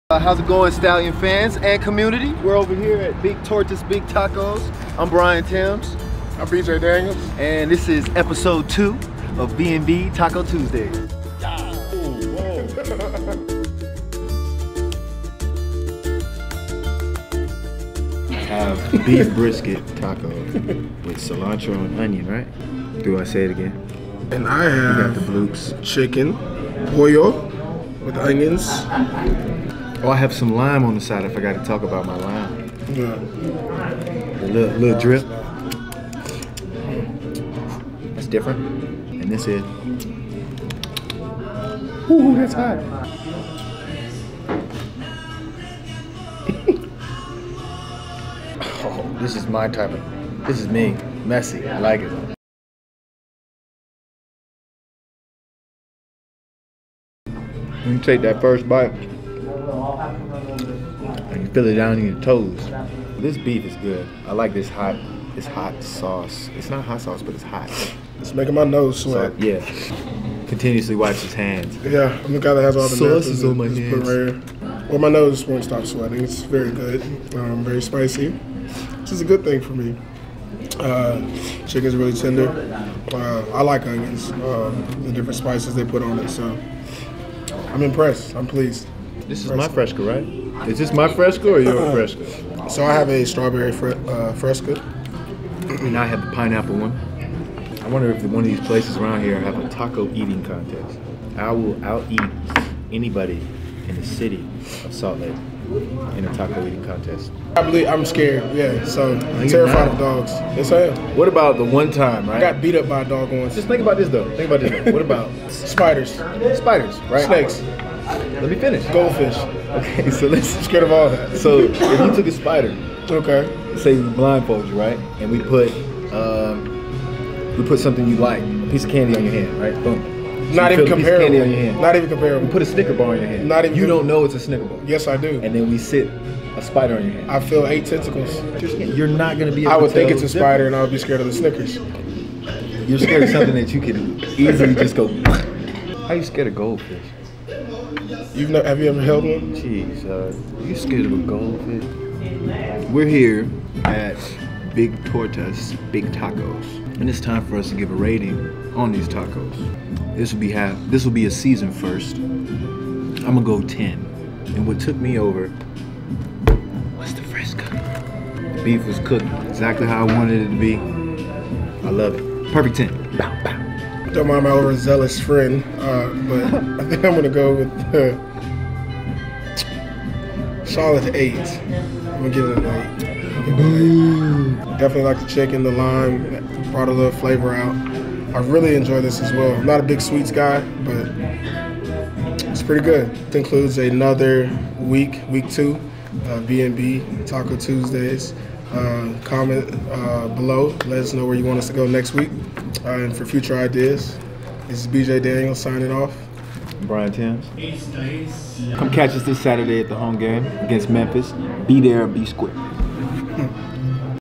B&B Taco Tuesday
Episode #2 of B&B Taco Tuesday, your weekly taco review with Stallions WR Brian Tyms and QB BJ Daniels. Leave in the comments below where you think they should head next on their search for the best taco/burrito in Salt Lake City.